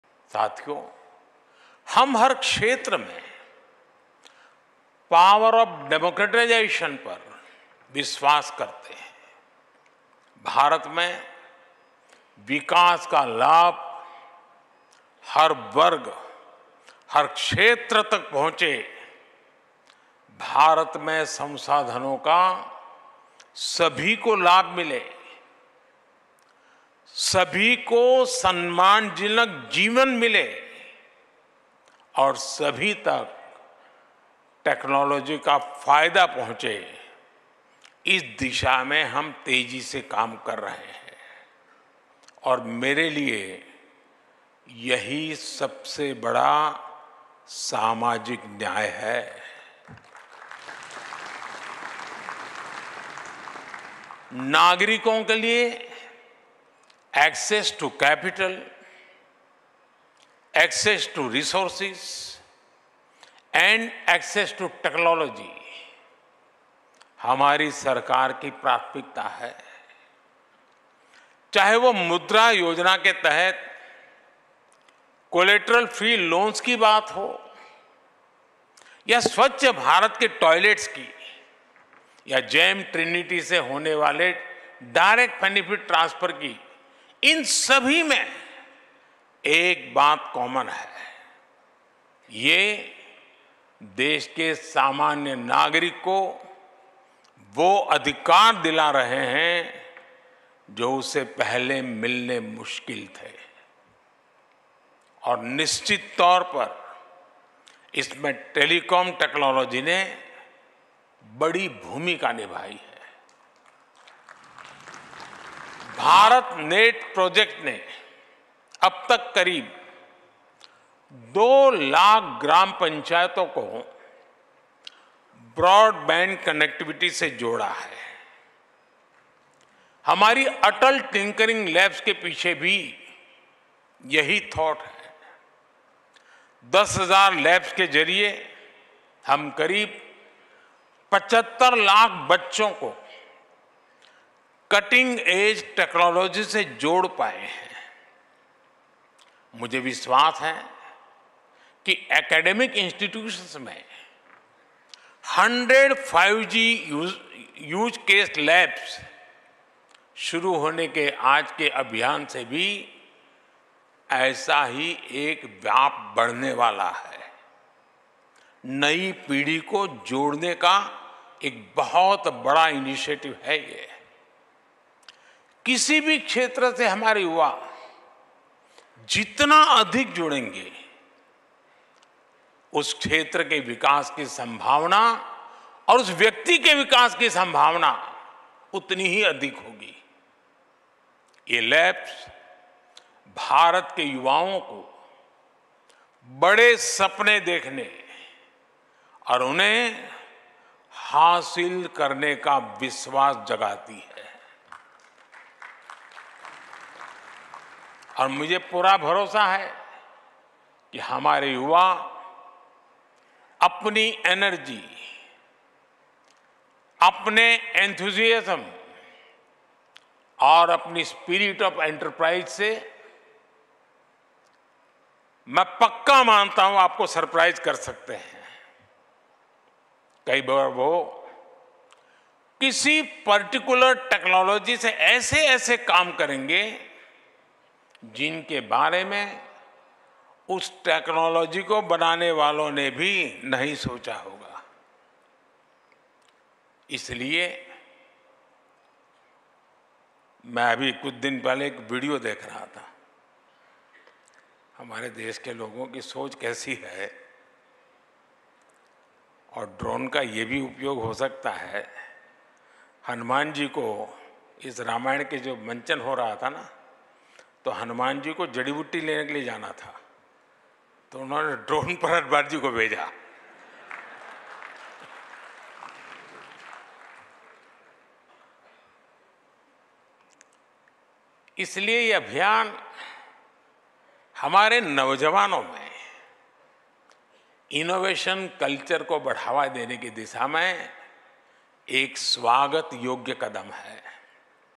Our Model of Governance has Enabled Access to Capital, Resources & Technology: PM Modi
PM Modi inaugurated the 7th Edition of the India Mobile Congress 2023 at Bharat Mandapam in New Delhi. Addressing the gathering, the PM Modi said that in the changing times of the 21st century, this event has the power to change the lives of crores of people. Underling the fast pace of technology, the PM Modi said “The future is here and now”.